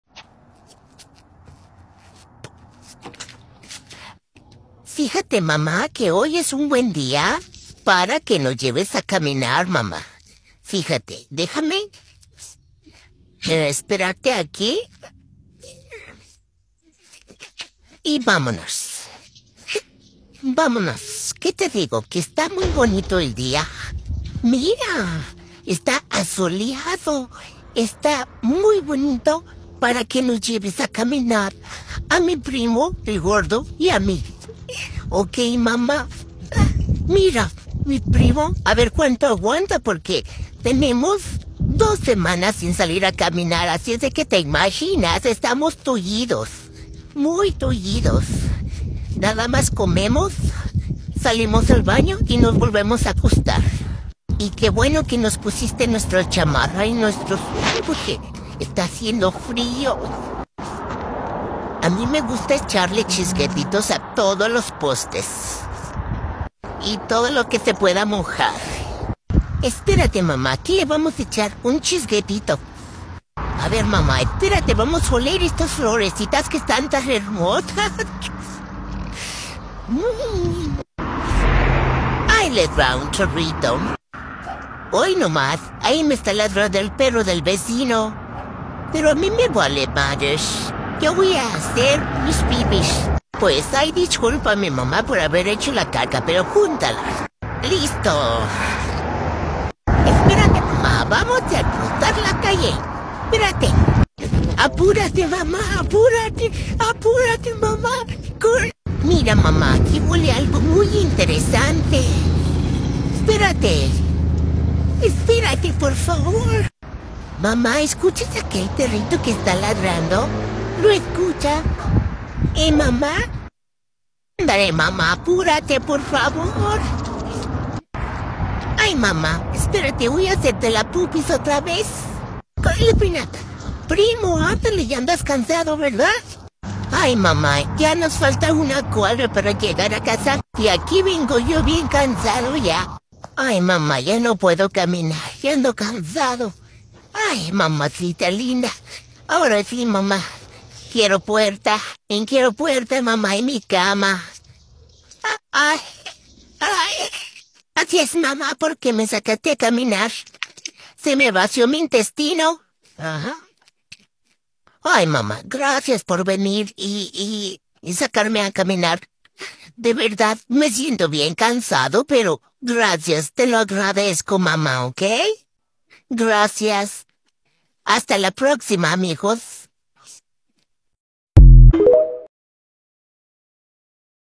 Los perritos nos sacaron a caminar!!
Cc sacas a caminar a tus perritos y ellos te llevan por donde ellos quieren!! @chelorodriguez6288